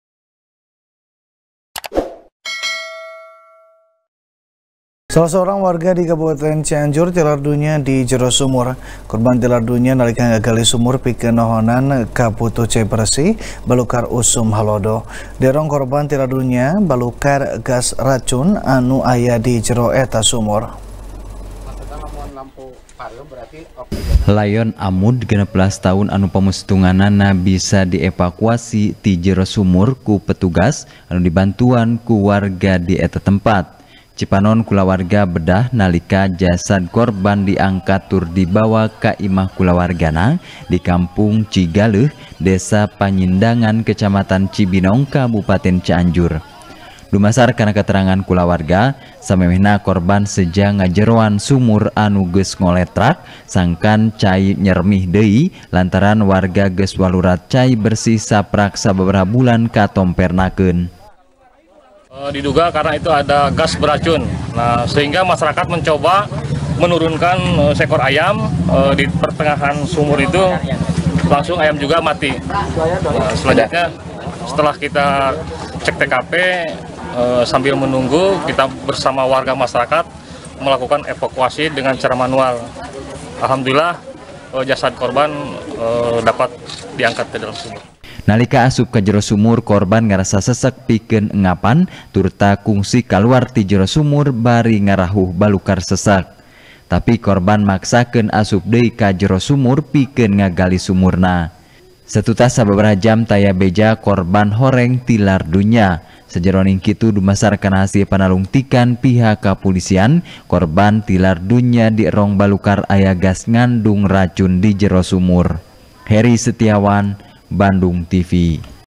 Dierong Balukar Gas Racun, Warga Tilar Dunya Nalika Ngagali Sumur | TP | BANDUNGTV
TP | BANDUNGTV | DERI FIRMAN | Salah Saurang Warga Di Kabupaten Cianjur, Tilar Dunya Di Jero Sumur. Korban Tilar Dunya Nalika Ngagali Sumur Pikeun  Nohonan Kabutuh Cai Beresih Balukar Usum Halodo. Dierong Korban Tilar Dunya Balukar Gas Racun Anu Aya Di Jero Eta Sumur.